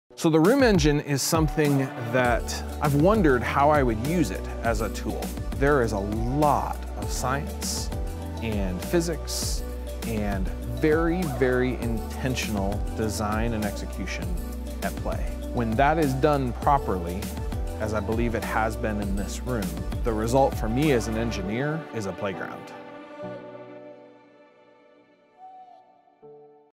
Mount Paran Church | L-Acoustics Room Engine
Diversified's Tim Corder discusses the use of L-Acoustics Room Engine at Mount Paran Church in Atlanta, GA.